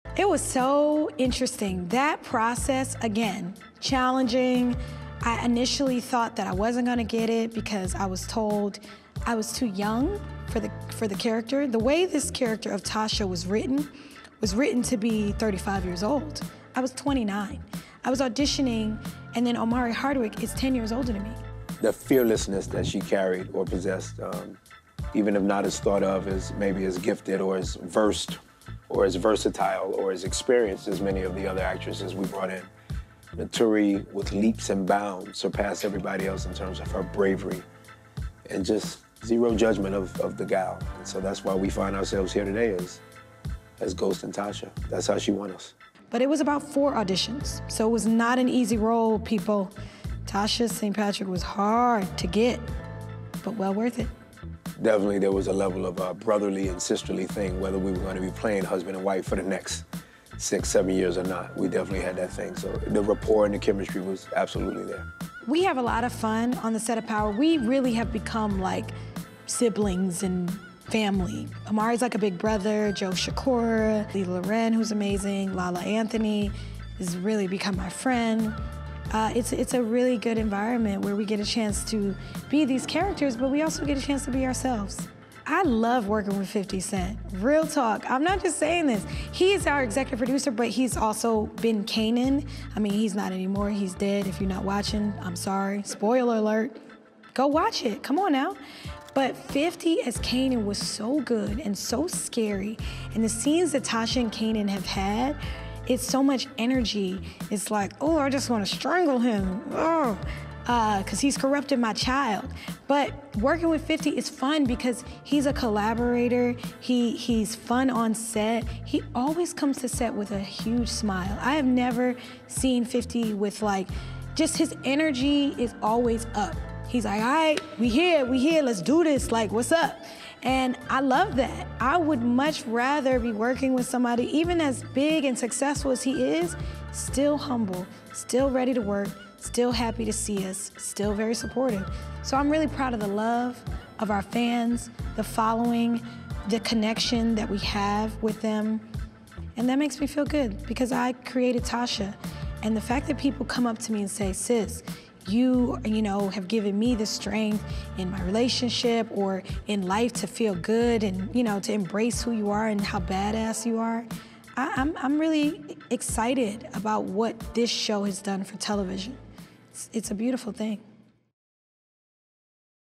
How Naturi Naughton Was Chosen As Tasha St. Patrick in Power | Uncensored
Power's Naturi Naughton & Omari Hardwick reveal the process of auditioning for her role and life on set with the cast.  for more.